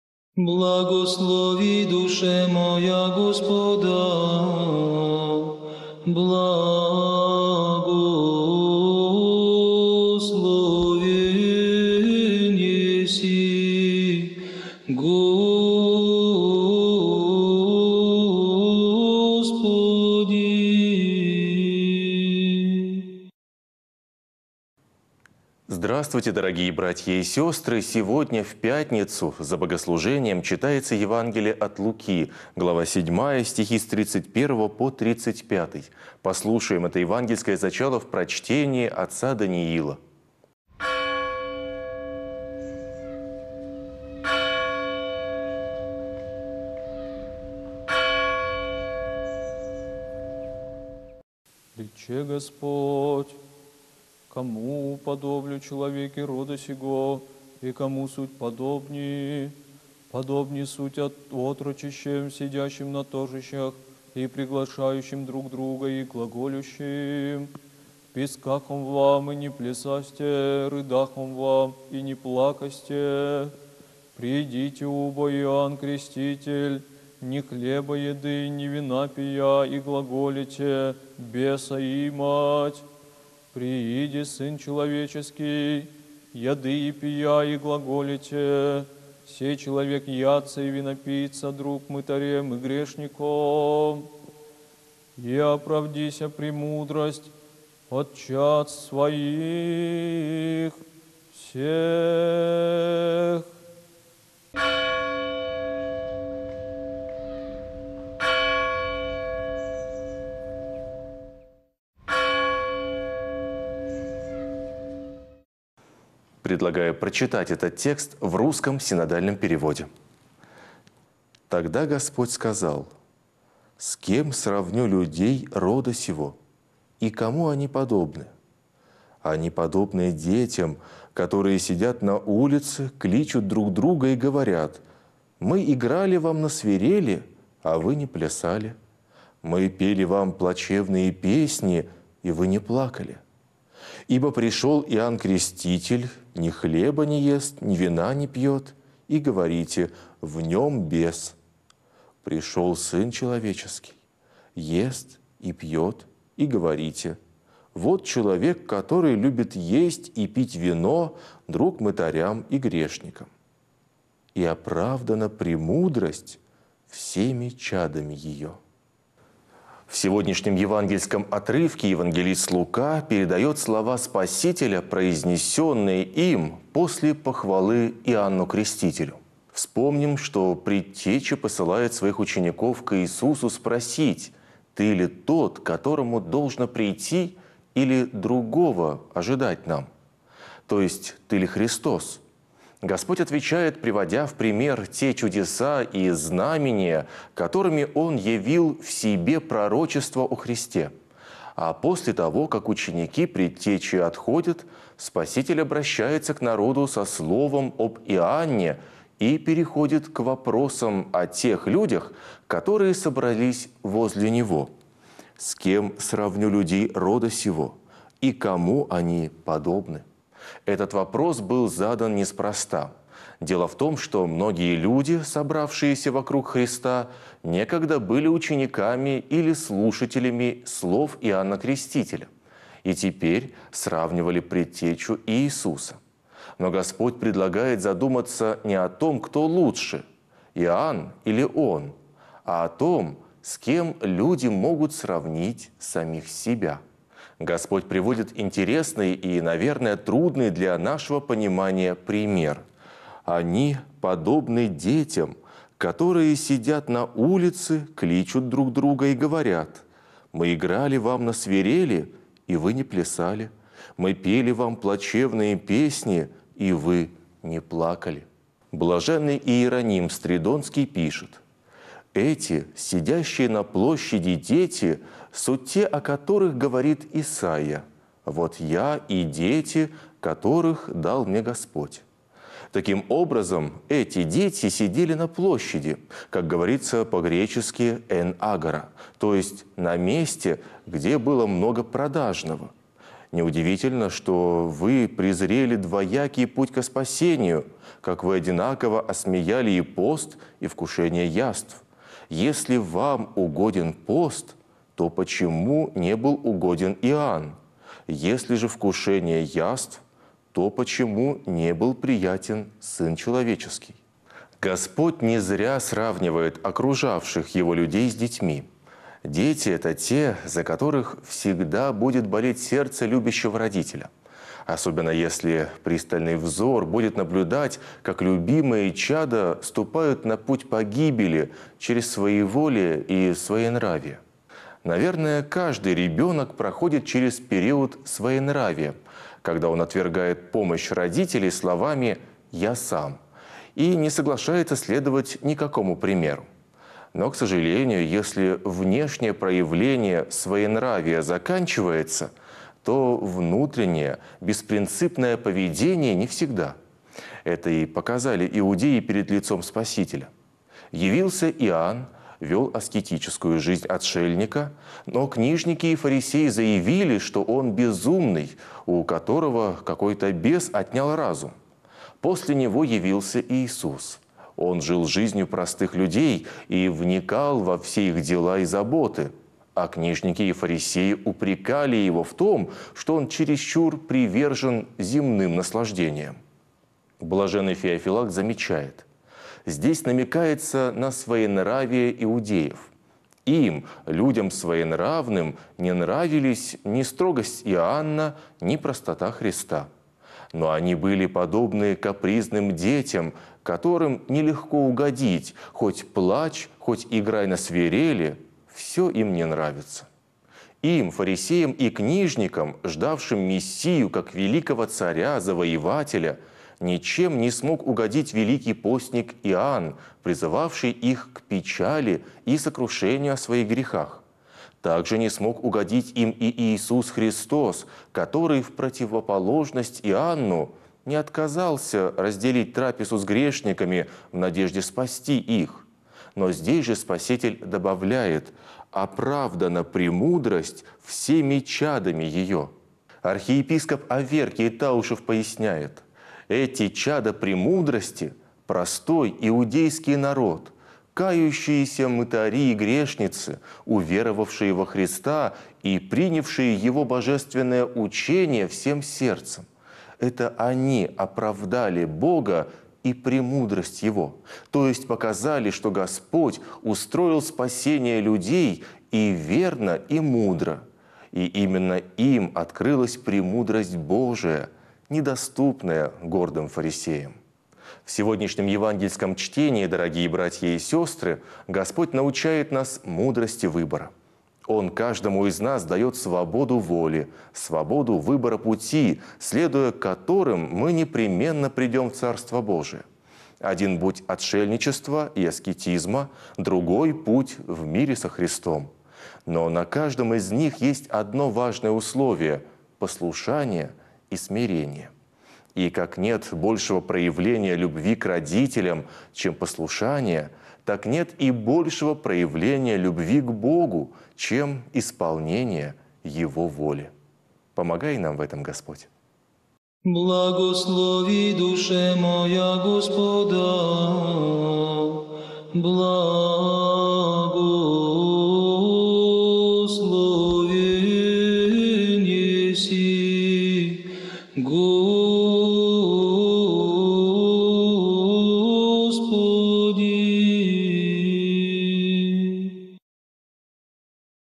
Читаем Евангелие вместе с Церковью 20 октября 2017г. Евангелие от Луки. Глава 7, ст. 31–35.
Тогда Господь сказал: с кем сравню людей рода сего? и кому они подобны?